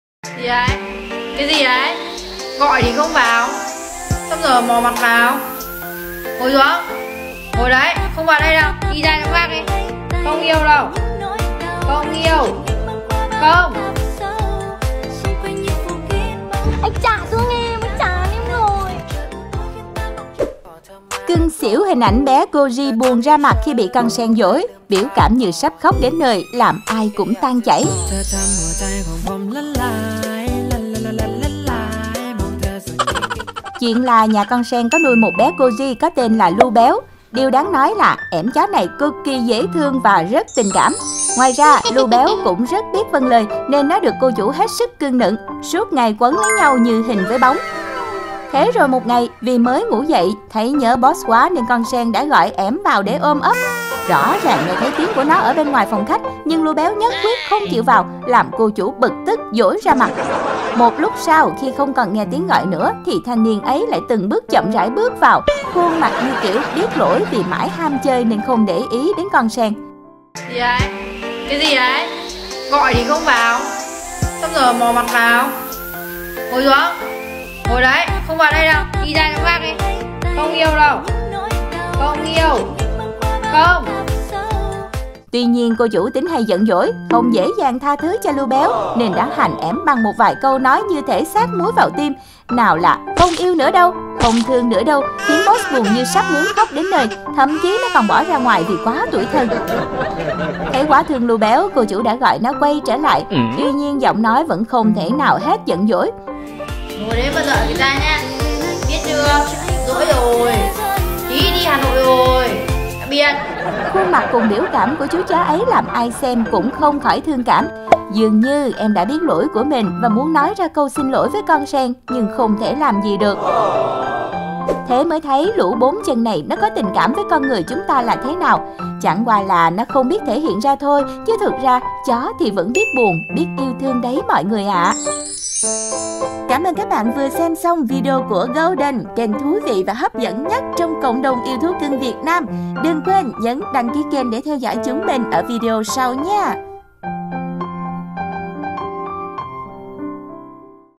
Cưng xỉu hình ảnh Bé Corgi 'BUỒN RA MẶT' khi bị con Sen 'HỜN DỖI' làm ai cũng tan chảy 🥰 Gâu Đần
🐶Gâu Đần🐶